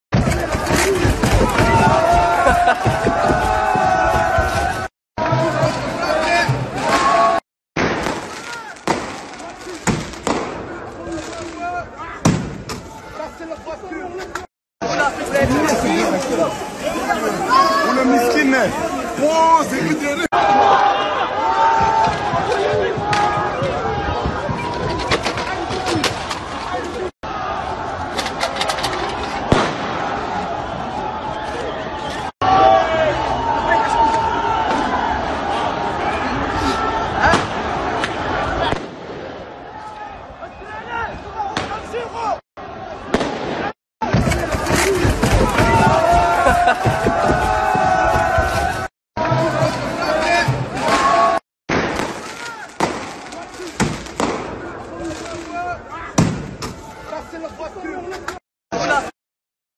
A Bruxelles i tifosi del Marocco assaltano le vie del centro per festeggiare la vittoria contro...
Guerriglia urbane nella capitale belga dopo il successo ai Mondiali

 Guerriglia urbana a Bruxelles dopo la vittoria del Marocco contro il Belgio ai Mondiali di calcio in Qatar. Alcune decine di giovani tifosi del Marocco hanno assaltato le vie del centro città e l'area adiacente alla stazione di Midi, distruggendo l'arredo urbano e appiccando il fuoco ad auto, scooter e monopattini elettrici.
Sul posto sono intervenuti un centinaio di agenti della polizia in divisa anti-sommossa con camionette e idranti. Gli scontri sono tuttora in corso. Boulevard Anspach, il principale viale del centro, è stato chiuso e la polizia ha fatto appello ad evitare la zona. ( Corriere Tv ). Guarda il video su Corriere